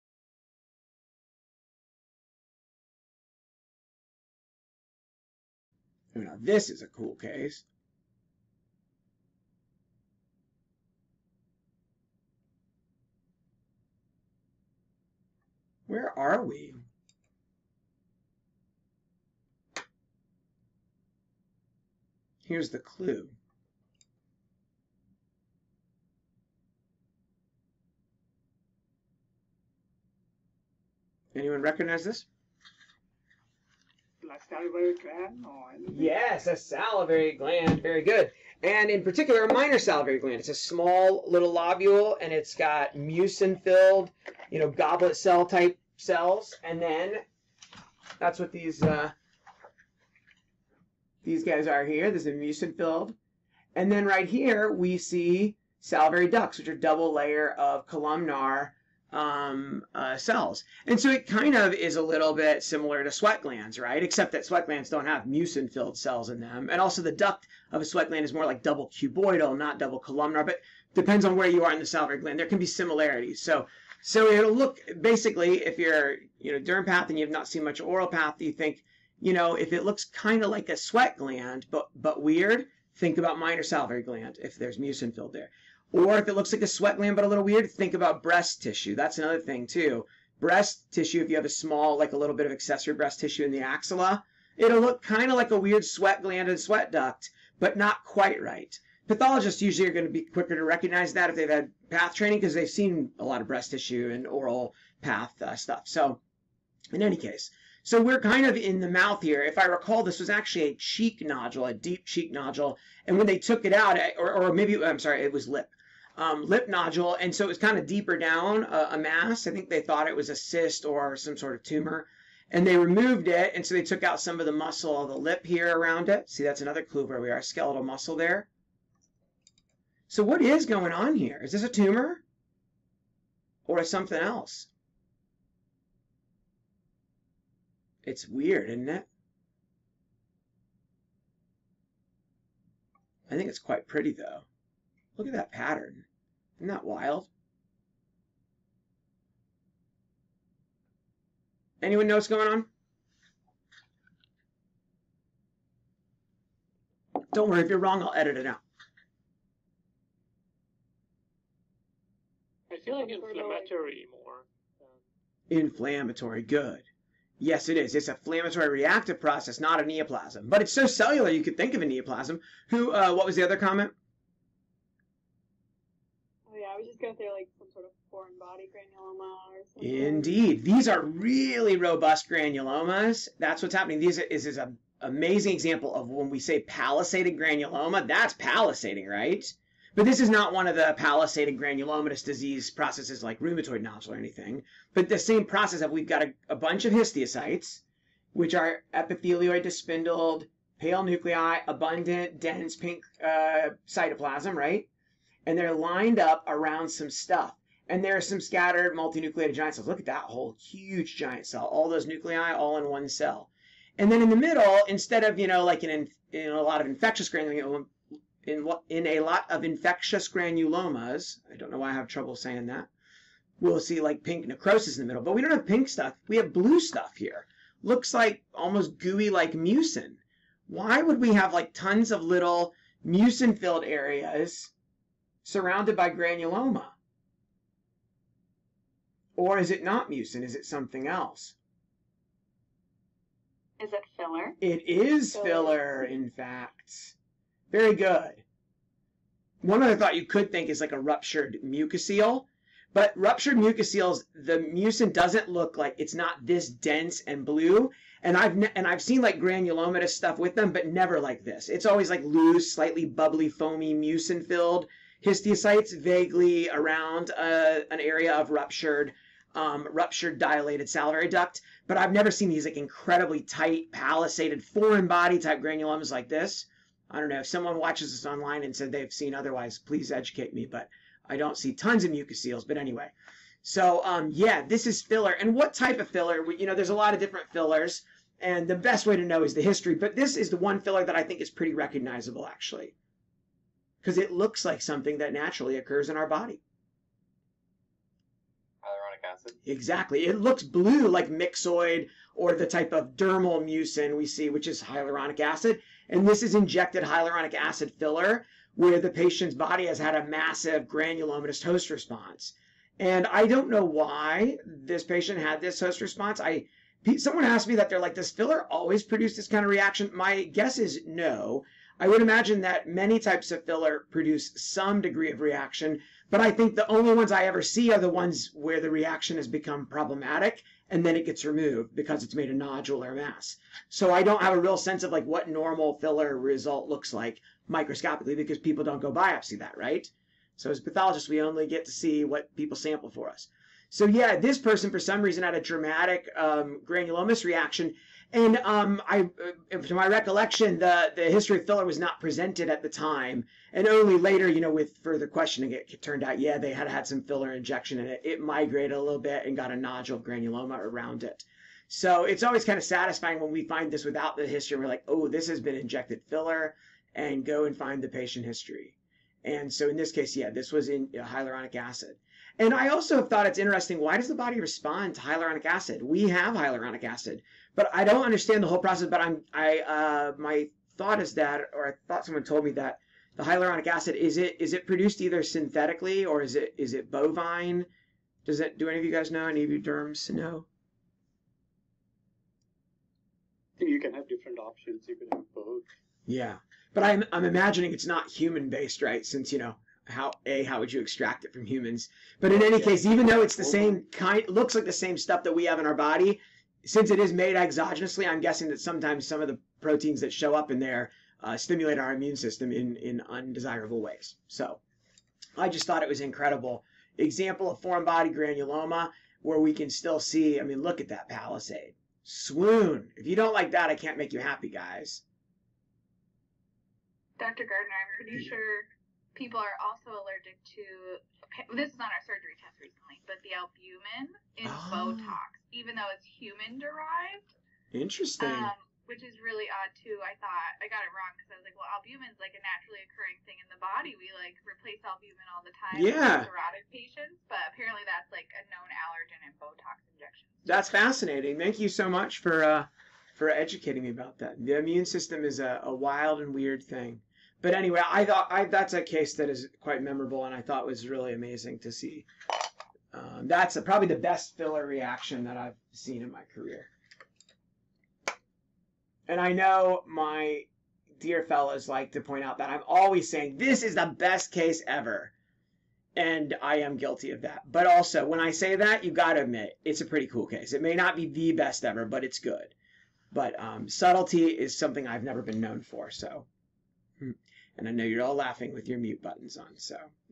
Reaction to Skin Fillers (Hyaluronic acid lip filler with palisaded granuloma reaction)
Excerpt from a dermatology and dermpath board review (full video with my residents and fellows. 17 cases in 1.25 hours. Enjoy!